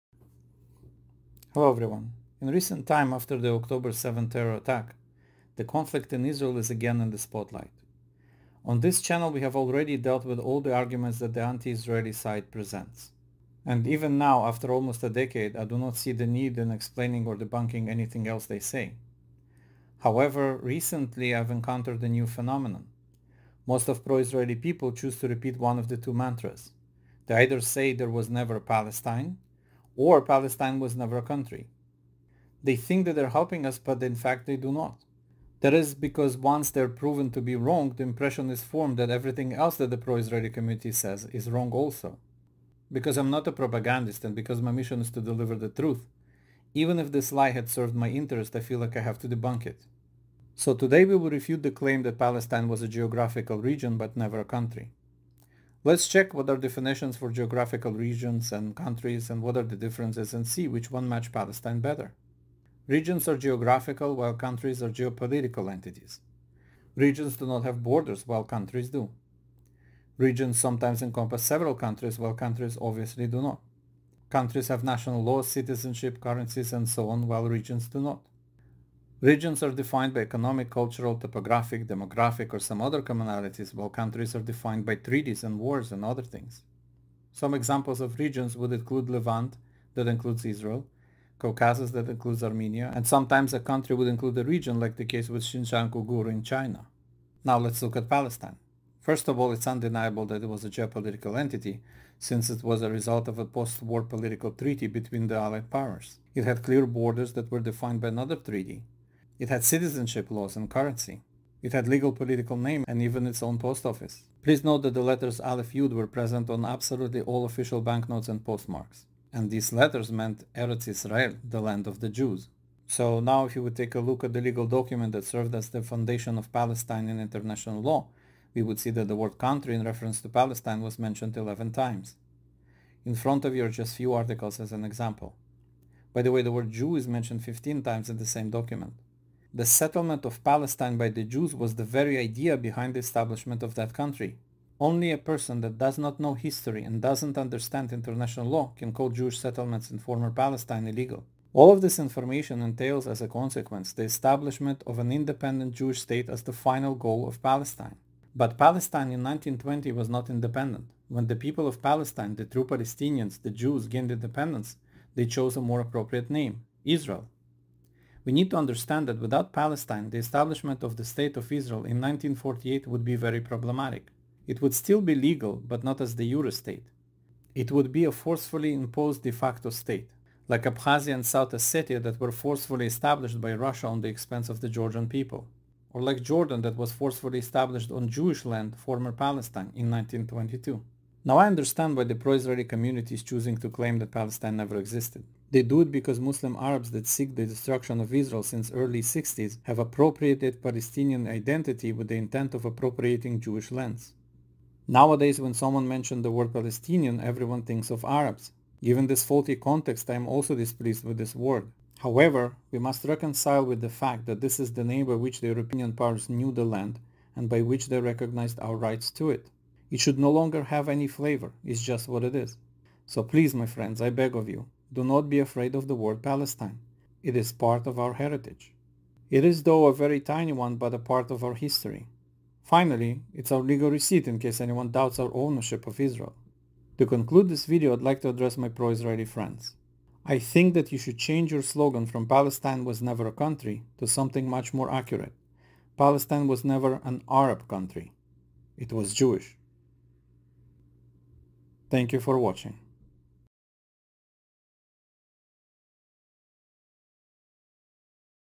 Stolen : Chapter XII : Was Palestine a Country?
This is the 12th video in a series of videos that are dealing with the legal foundation of Israel in terms of International Law.